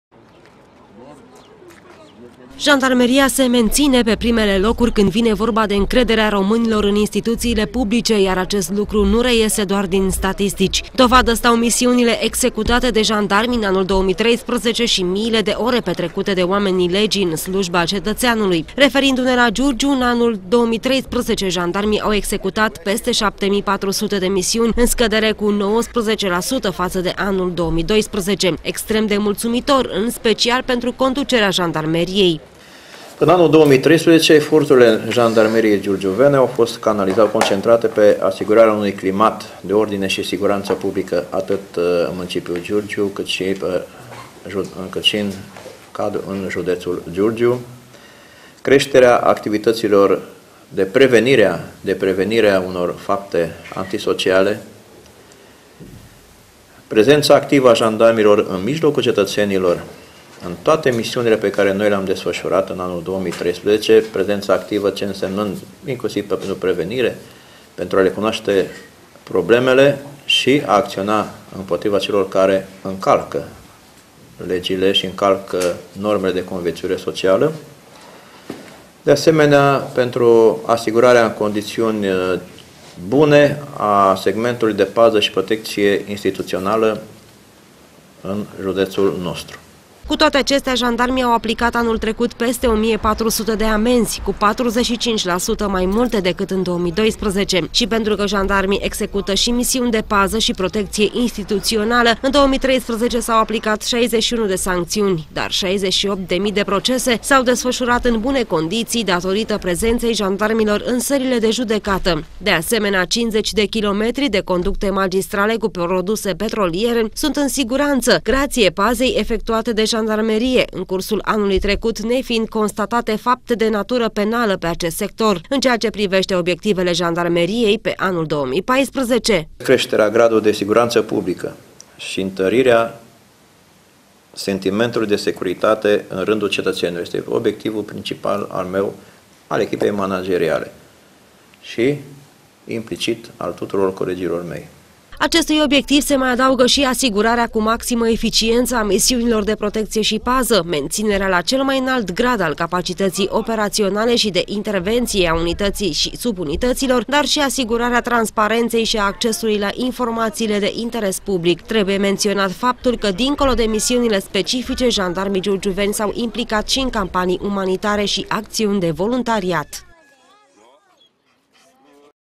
2013, AN CU REZULTATE PENTRU JANDARMI
Bilanț pozitiv al anului 2013 pentru jandarmeria giurgiuveană. Deși s-au executat circa 7.500 de misiuni, infracționalitatea este mai mică decât în 2012, semn că prezența jandarmilor printre giurgiuveni este de bun augur. Cele mai multe misiuni au fost cele de menținere a ordinii publice, toate încheiate cu succes, fără victime sau alte evenimente negative.